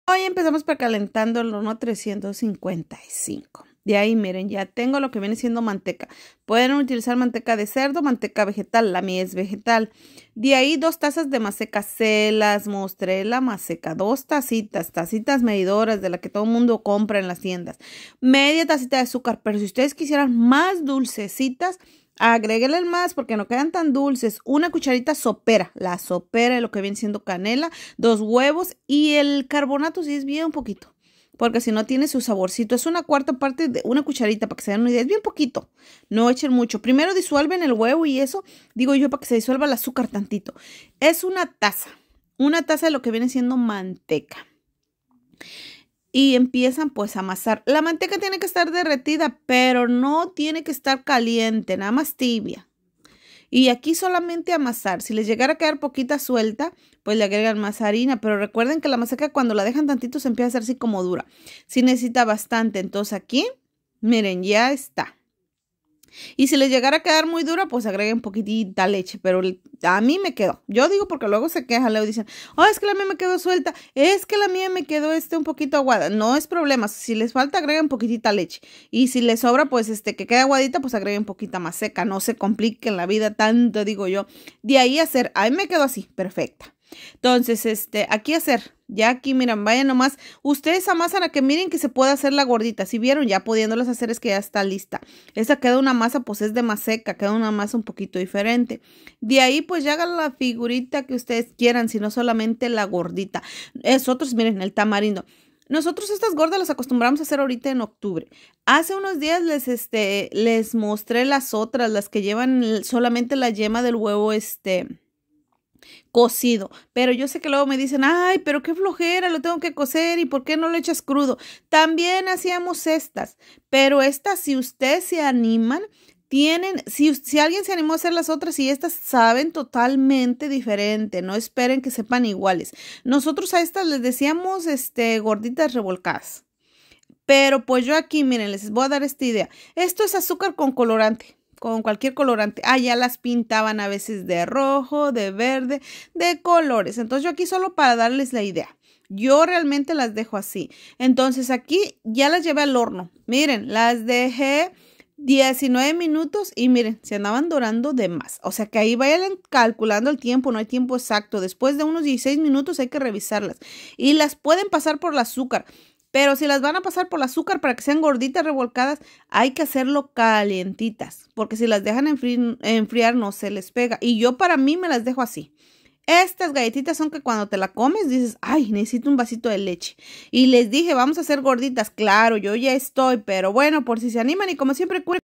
Es Octubre hacer Gorditas para las ofrendas una idea mas facil
chicas esta Gorditas quedan deliciosas tambiem una idea mas facil de hacerlas 😋